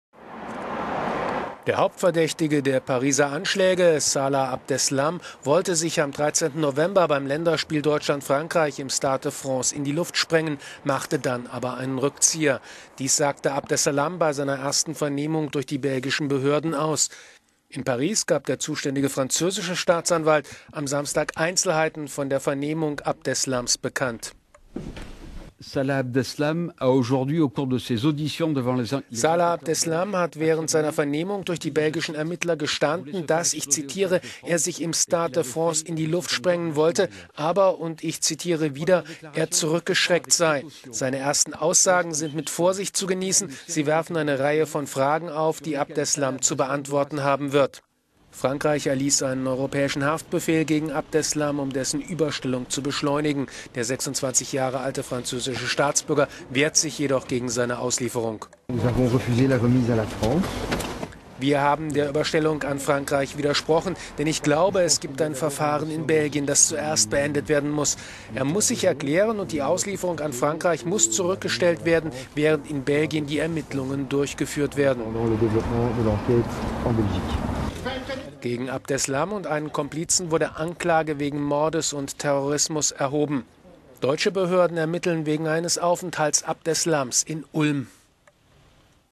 Abdeslam sagt aus: „Ich wollte mich in Paris in die Luft sprengen“
Der Hauptverdächtige der Pariser Anschläge, Salah Abdeslam, wollte sich am 13. November bei dem Länderspiel Deutschland-Frankreich im Stade de France in die Luft sprengen, machte dann aber einen Rückzieher. Dies sagte Abdeslam bei seiner Vernehmung durch die belgischen Behörden aus. In Paris gab der zuständige französische Staatsanwalt François Molins am Samstag Einzelheiten von der Vernehmung Abdeslams bekannt.

"Salah Abdeslam hat während seiner Vernehmung durch die belgischen Ermittler gestan…